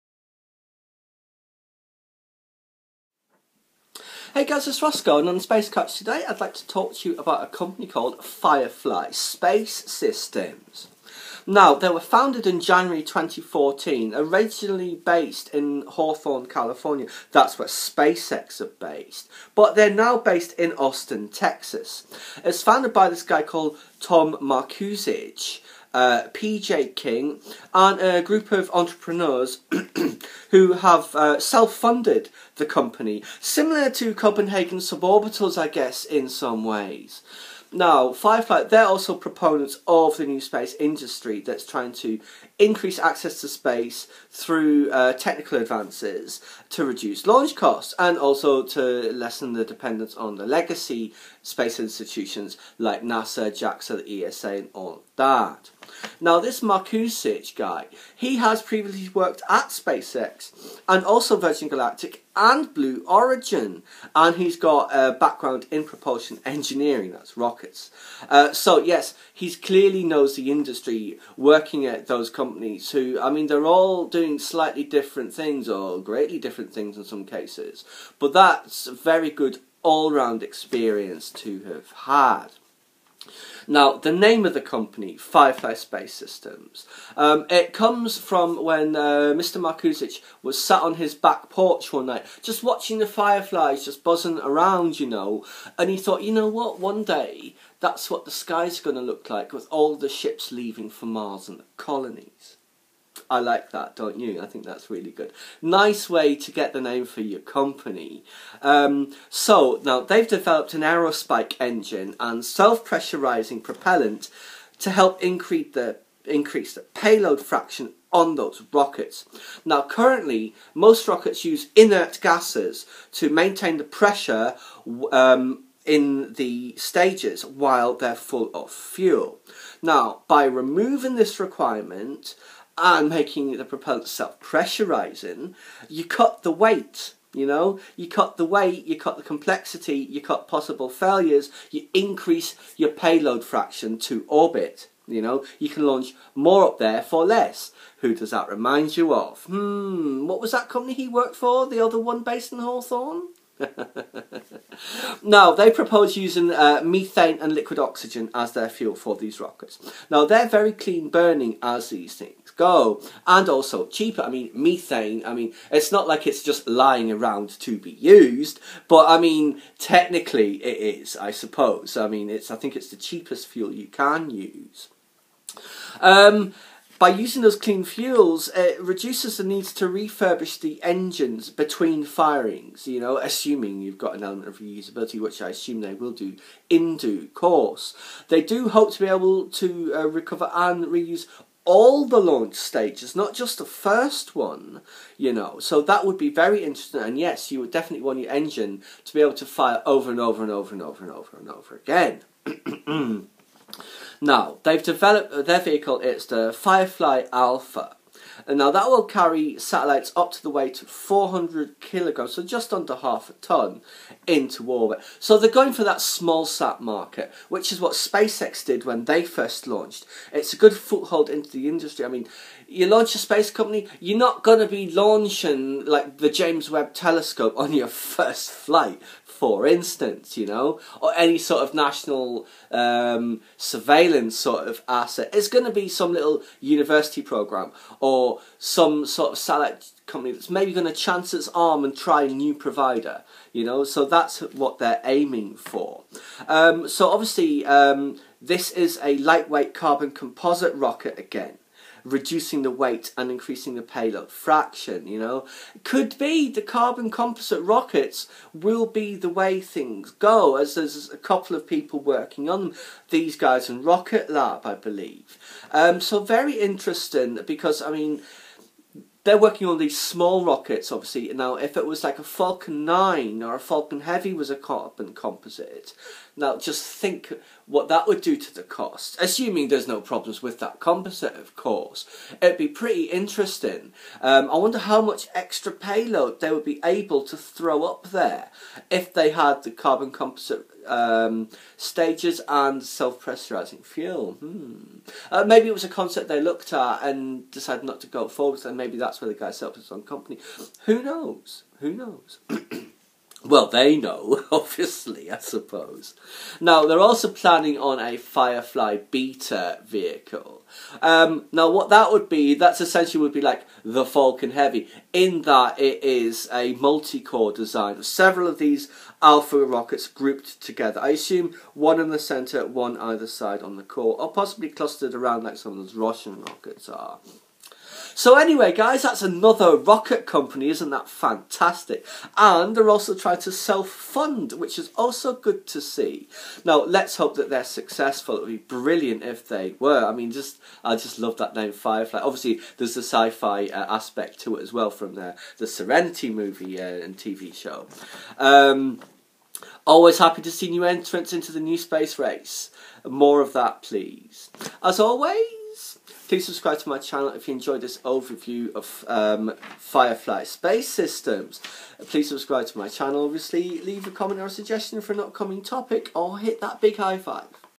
Firefly Space Systems - a Space Couch overview!
Another of the NewSpace rocket companies, aiming at the small satellite market